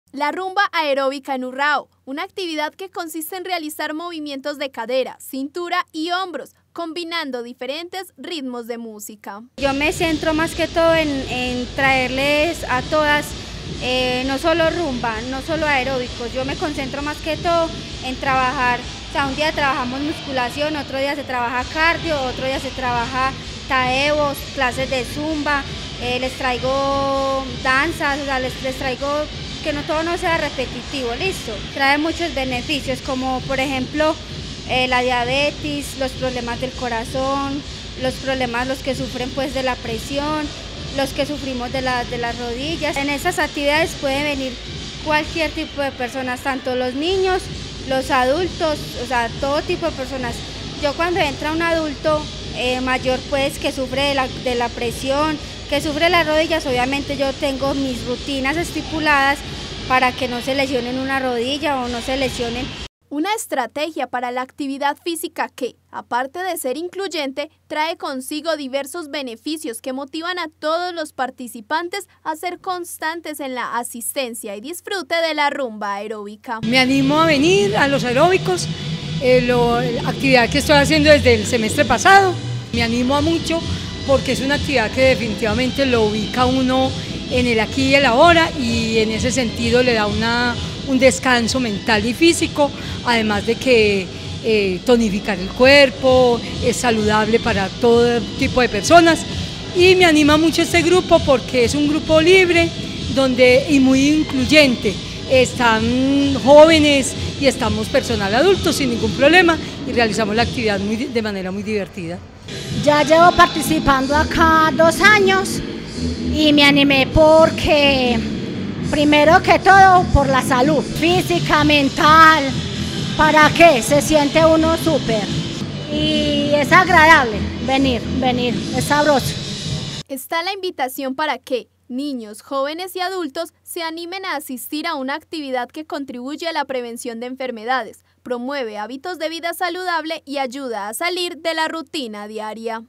¿Qué son las clases de rumba aeróbica?
Son varios espacios los que están abiertos al público para motivar a la comunidad urraeña a la realización de la actividadfísica. Uno de estos son las clases de rumbaaeróbica, veamos en qué consisten y la oportunidad que todos tienen de participar.